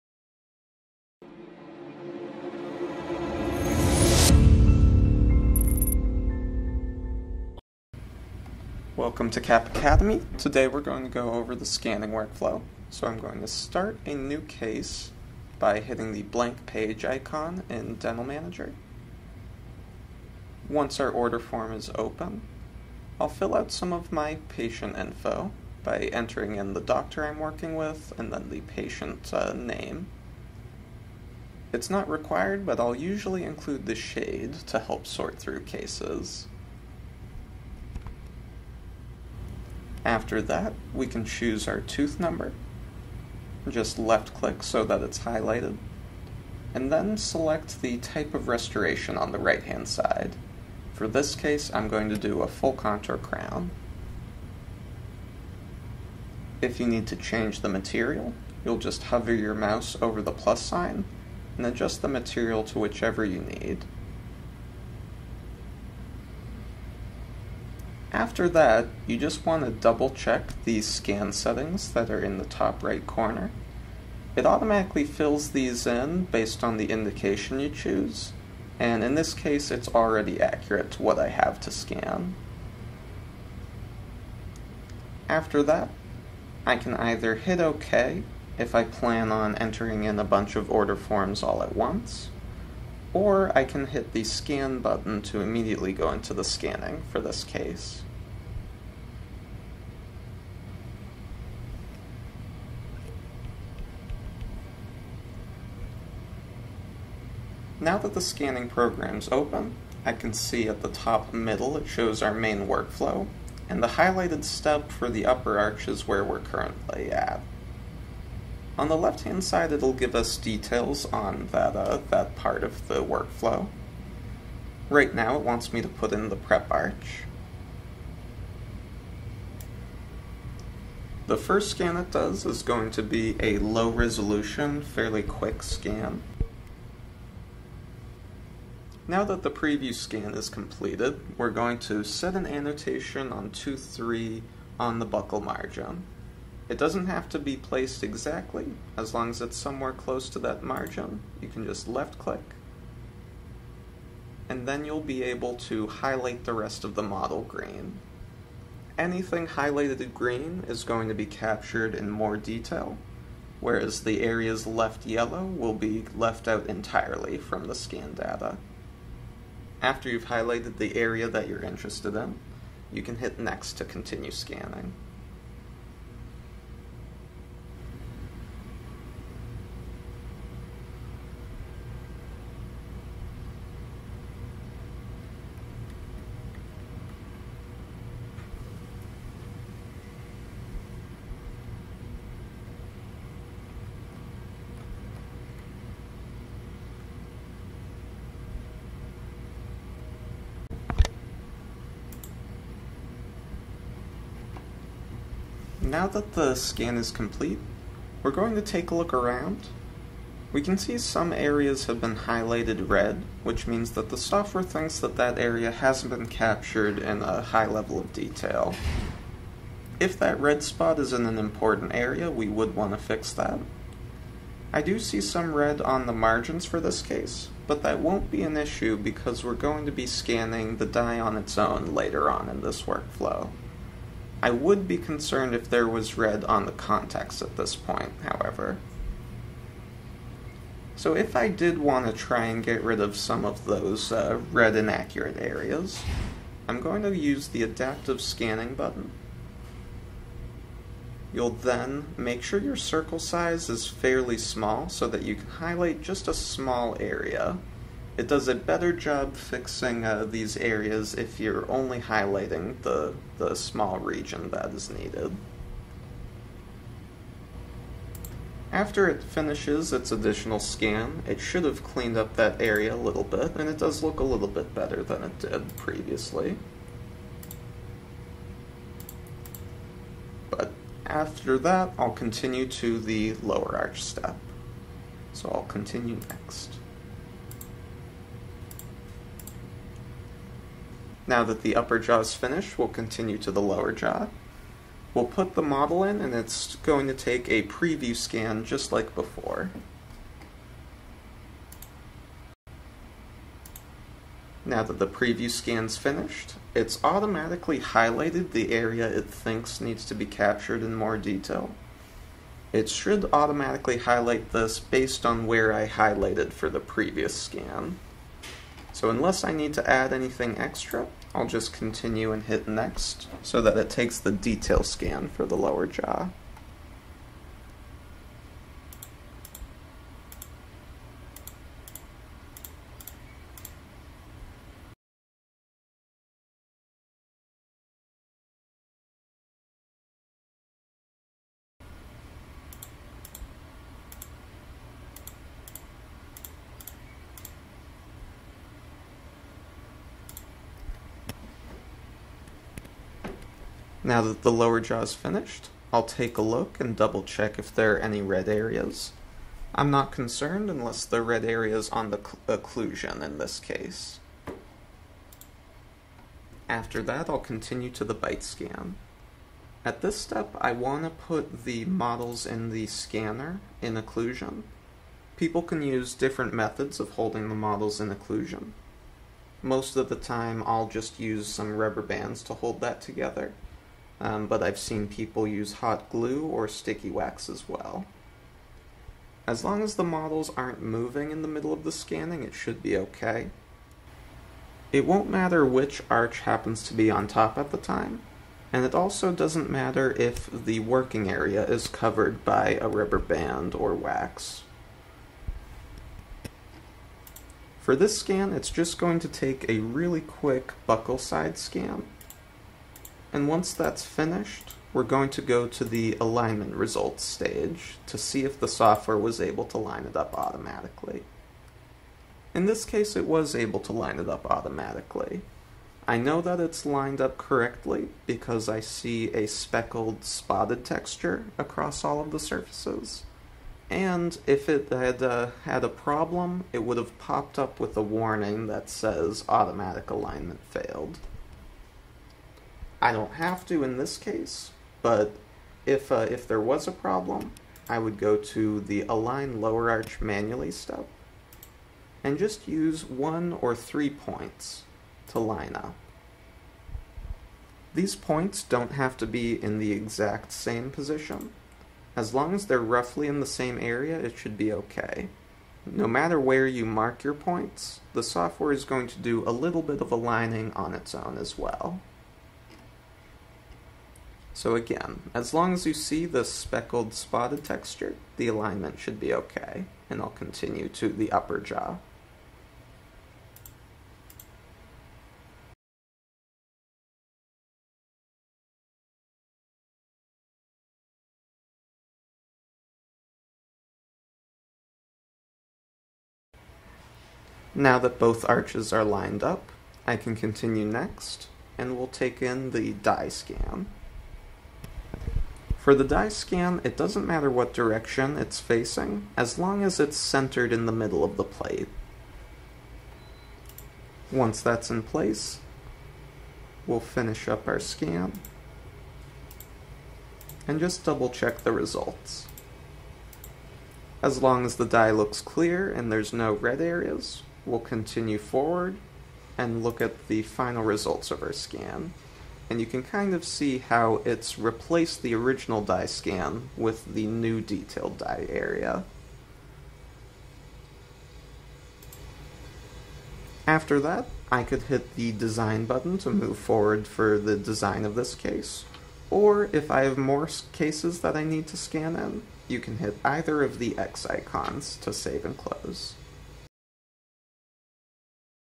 Dental Lab Life: Scanning a Crown in 3Shape
Alex de Vos, one of our Technical Support Specialists, uses a 3Shape scanner and the 3Shape software to show you the scanning workflow.